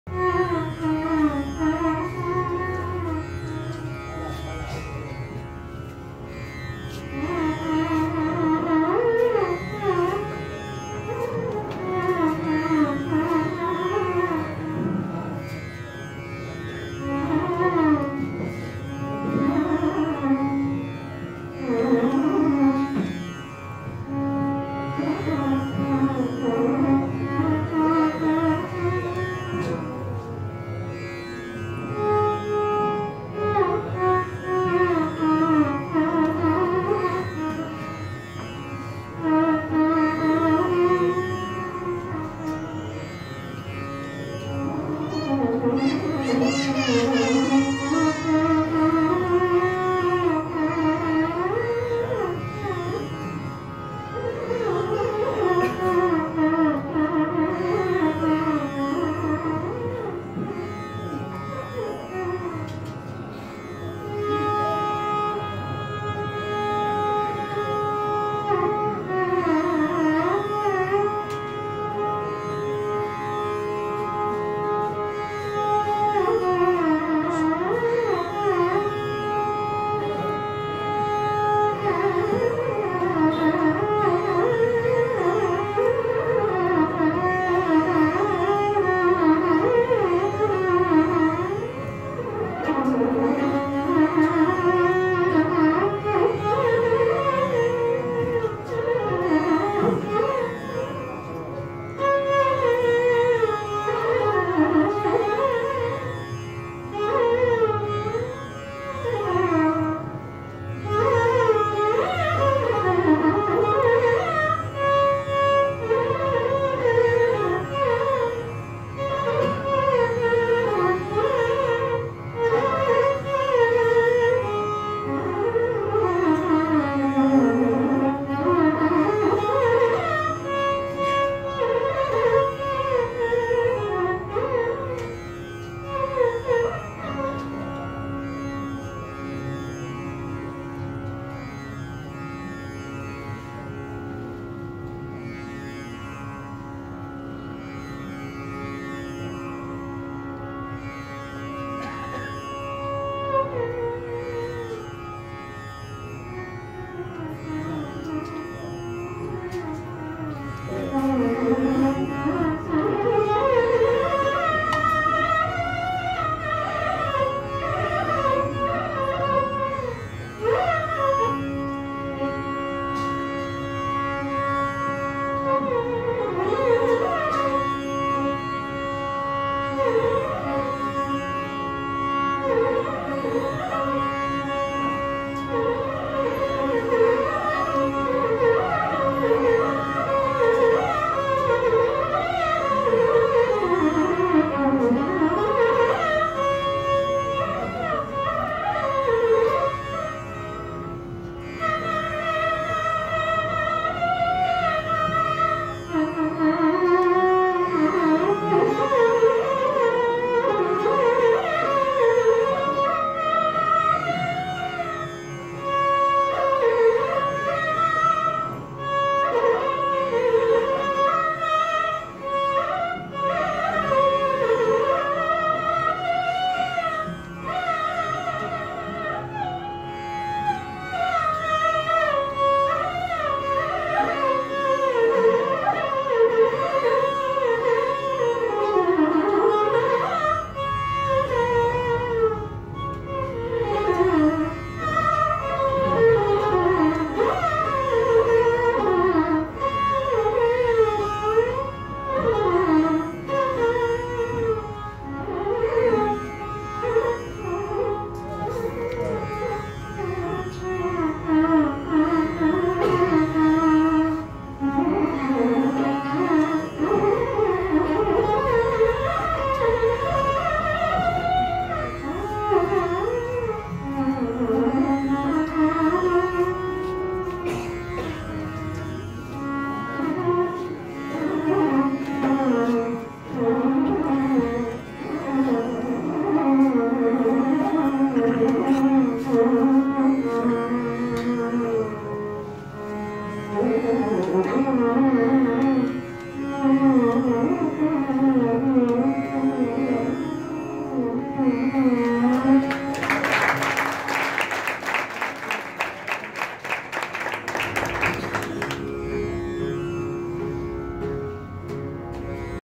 Vid. Nagai Sriram | Devagandhari Ragam
Concert at Raga Sudha Hall, Mylapore by Vid. Palghat Ramprasad on 17/02/2019.

Vocal - Vid. Palghat Ramprasad
Violin - Vid. Nagai Sriram
Mridangam - Vid. Trichy Sankaran
Ghatam - Vid. V. Suresh